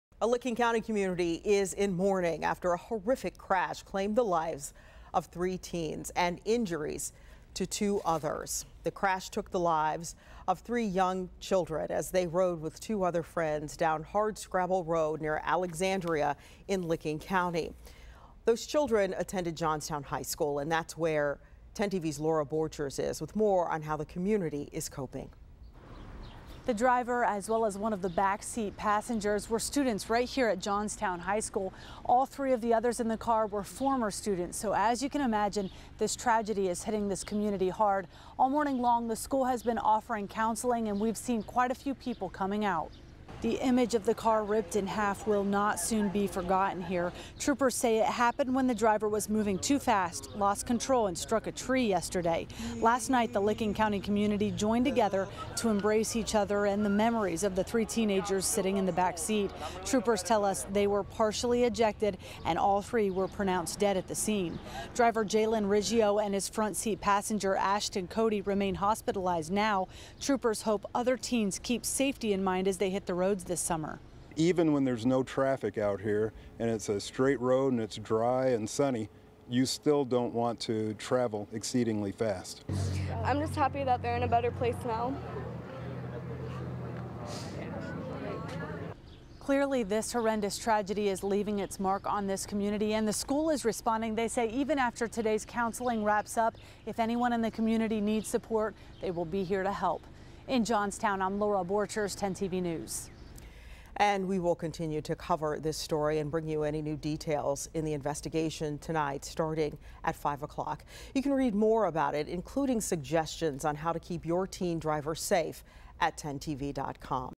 Tragic Crash
Community mourns the loss of three teens killed in crash